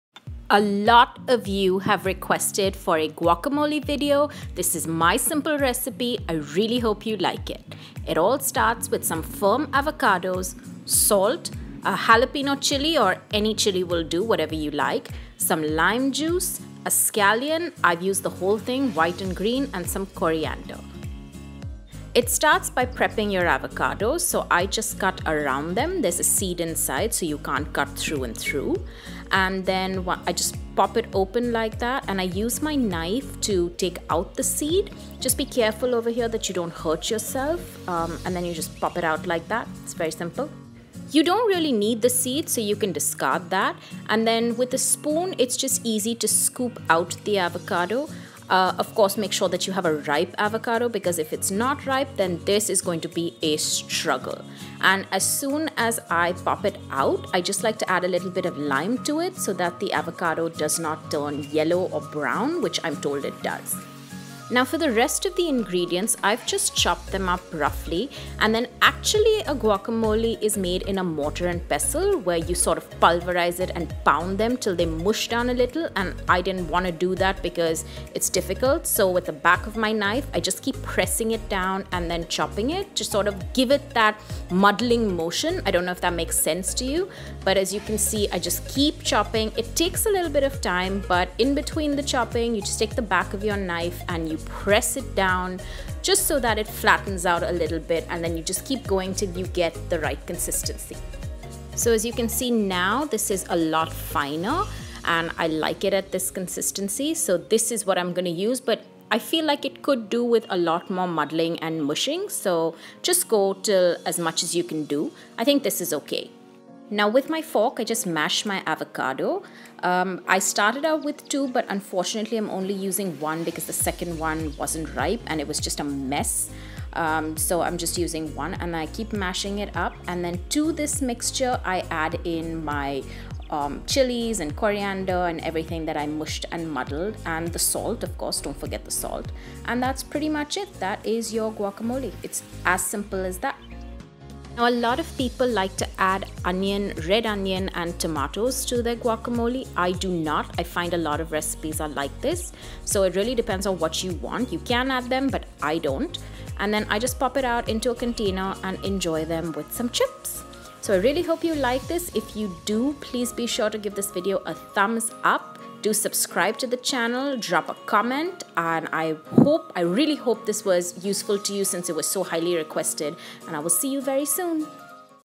The Tiny Taster | How To Make An Easy Home Made Guacamole Recipe
This is a super easy recipe for Guacamole. It was highly requested just like my Salsa recipe and I really hope you enjoy it. It's easy to make, lasts in the fridge well and can be used as an accompaniment in salads, with meats or eaten with chips as a guilty pleasure.

Hope you enjoy making this !

I would love to connect with you on social media, there are my links -